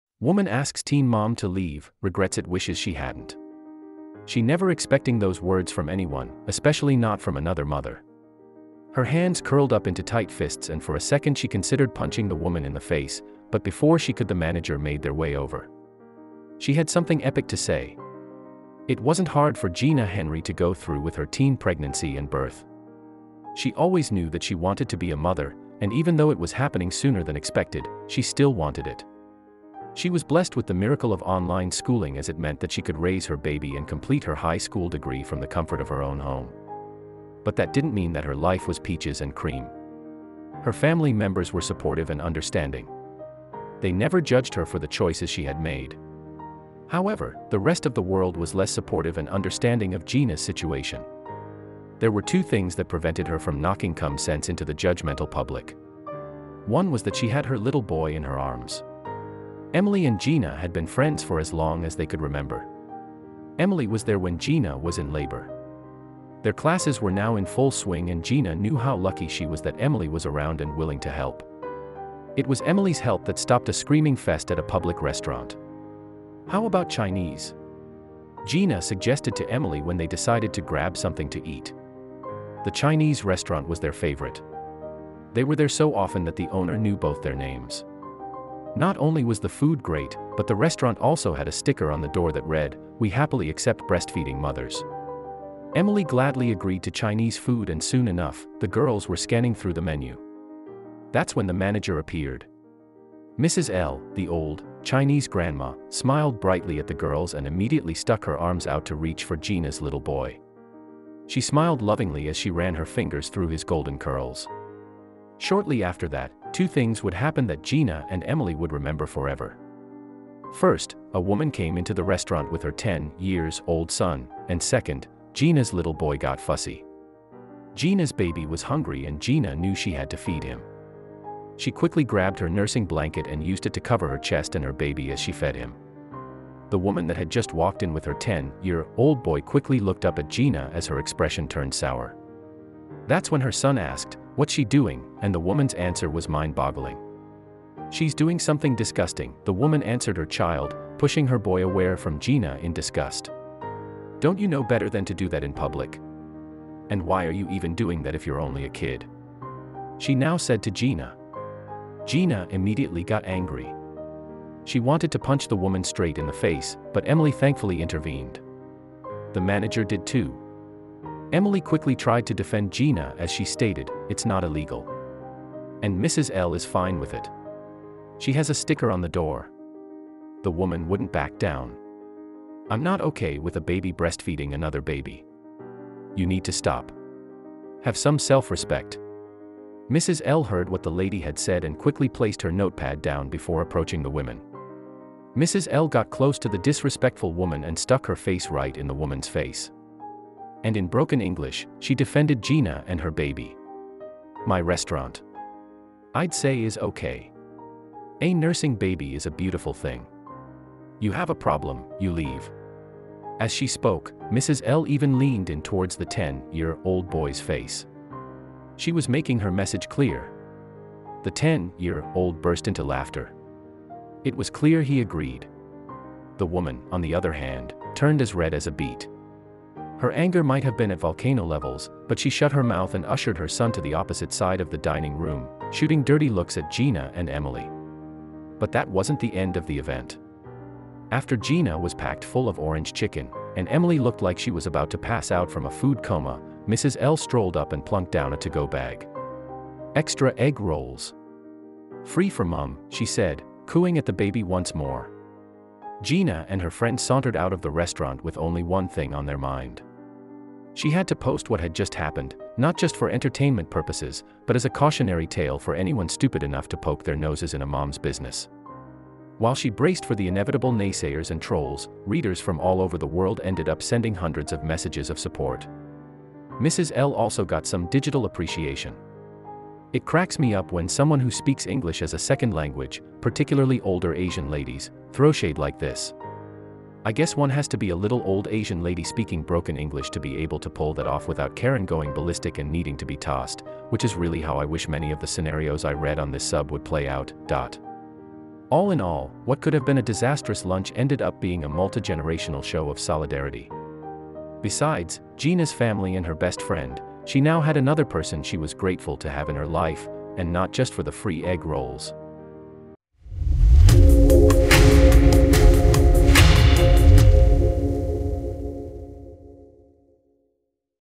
Woman Asks Teen Mom To Leave, Regrets It Wishes She Hadn't
She never expecting those words from anyone, especially not from another mother.

Her hands curled up into tight fists and for a second she considered punching the woman in the face, but before she could the manager made their way over. She had something epic to say.

It wasn't hard for Gina Henry to go through with her teen pregnancy and birth.

She always knew that she wanted to be a mother, and even though it was happening sooner than expected , she still wanted it. She was blessed with the miracle of online schooling as it meant that she could raise her baby and complete her high school degree from the comfort of her own home. But that didn't mean that her life was peaches and cream.

Her family members were supportive and understanding. They never judged her for the choices she had made. However, the rest of the world was less supportive and understanding of Gina's situation.